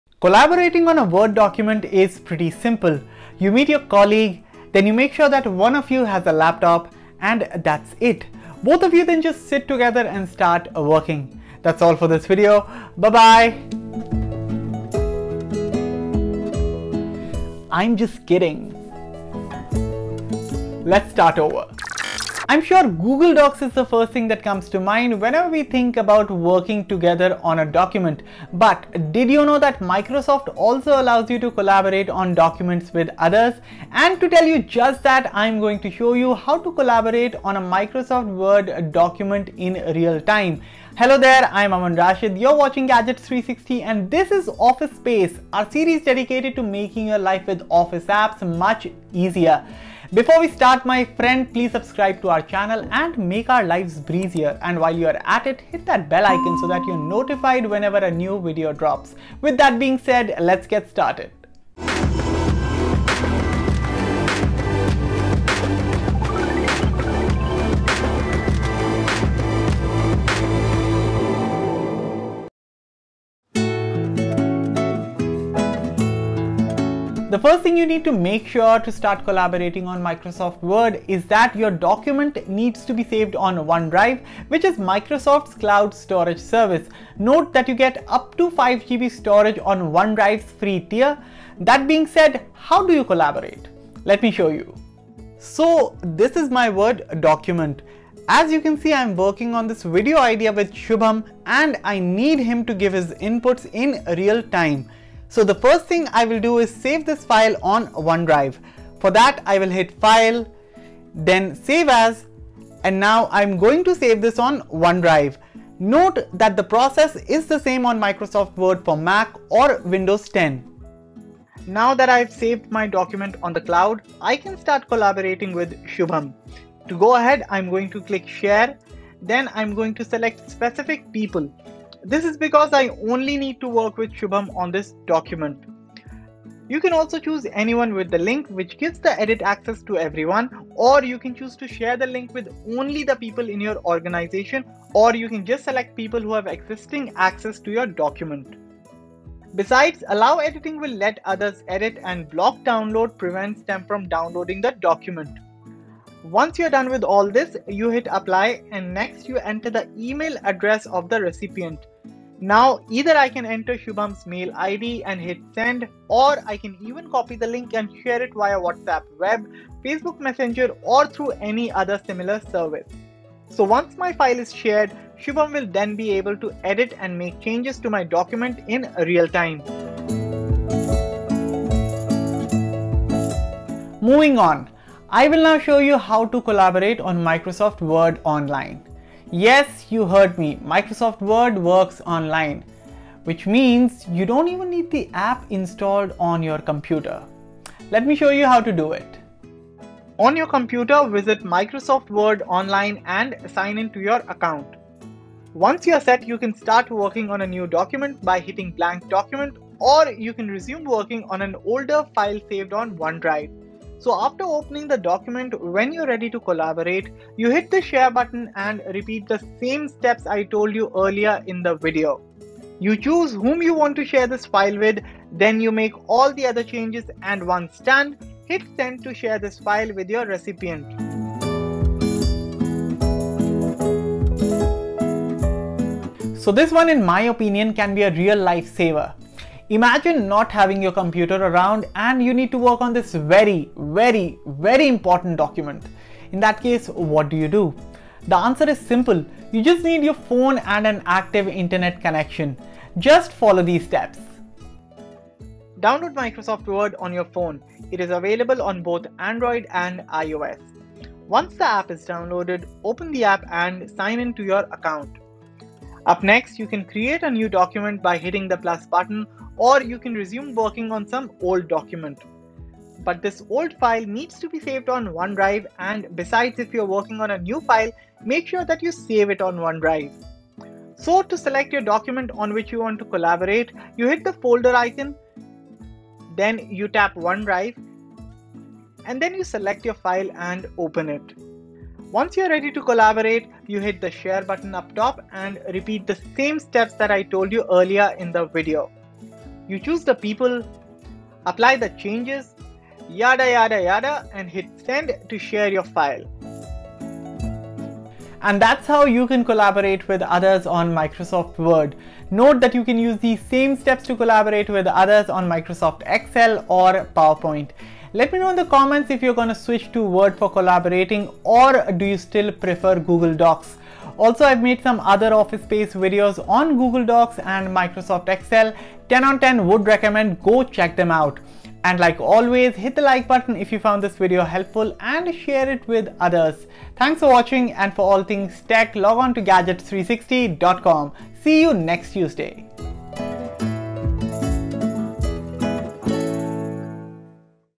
Microsoft Word Collaboration: How to Collaborate on Microsoft Word in Real Time
Whenever you think of collaborating on a document, Microsoft Word collaboration is not the first tool that comes to mind. However, did you know Microsoft Word also lets you collaborate with others on documents just like Google Docs? This is done by allowing users to invite others to collaborate so that the changes can be made while working on the document. In this video, I will show you how to collaborate on a Microsoft Word document in real time.

Welcome everyone, this is Office Space episode 3. For those who don't know, Office Space is our series dedicated to making your life with office apps much easier.

0:00 Introduction
1:13 Collaborate on a Computer
3:52 Collaborate on a Phone

Microsoft Word Collaboration: How to Collaborate on a Word Document on a Computer, Phone,